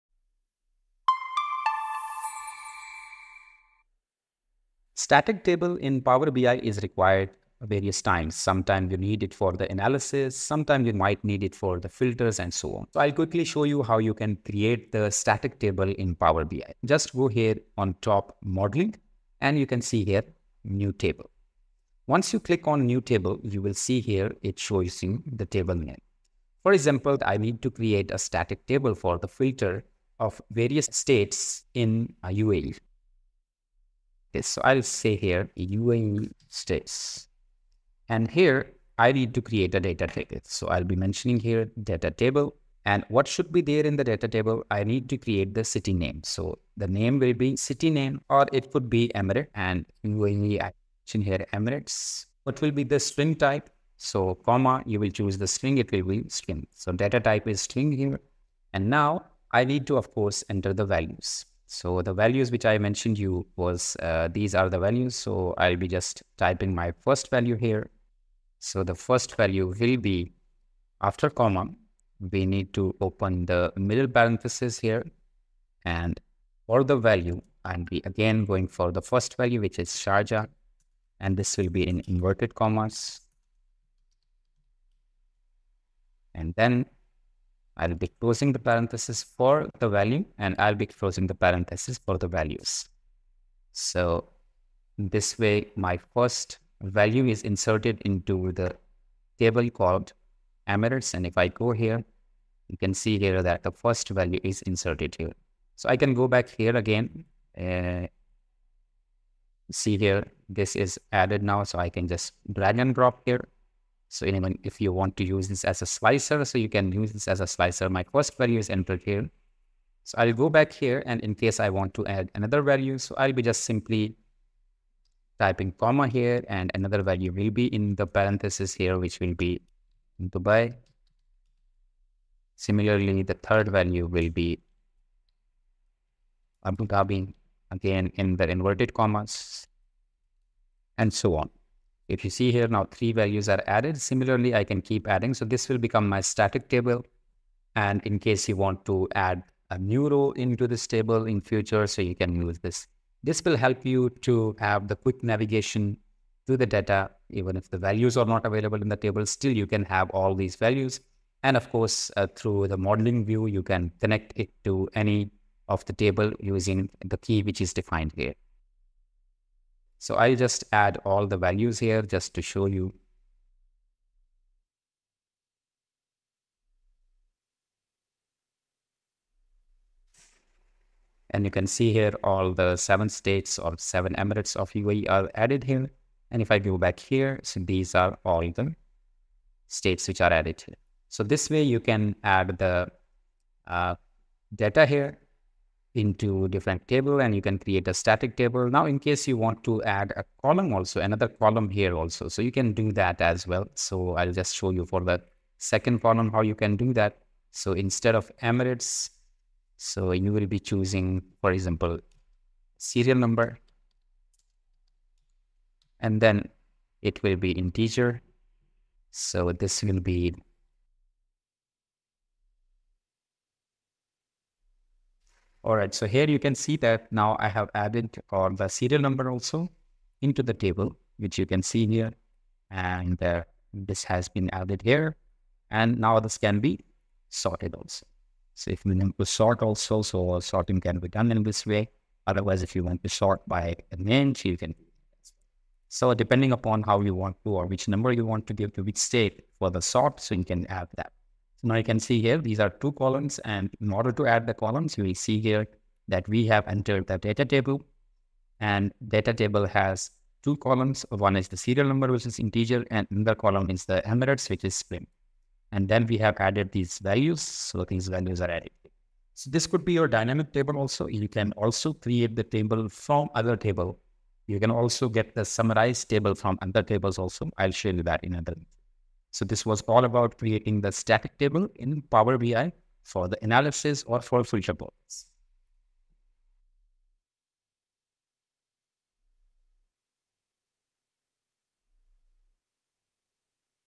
Master Power BI: Create Static Tables Like a Data Scientist | Step-by-Step Tutorial
In this comprehensive tutorial, you will learn how to create static tables in Power BI to facilitate data analysis and filtering Analysis. In this step-by-step tutorial, I'll guide you through the process of creating a static table for analysis with a practical example. Once you complete this video you will have a good understanding of how to define columns, enter data, and even add sorting options to your static table. Whether you're a beginner or looking to enhance your Power BI skills, this tutorial has you covered. Don't miss out on this valuable Power BI training!

To Enroll for the Full course and Master your PowerBI as continue your journey of becoming a Data Scientist. The full Course Link is below;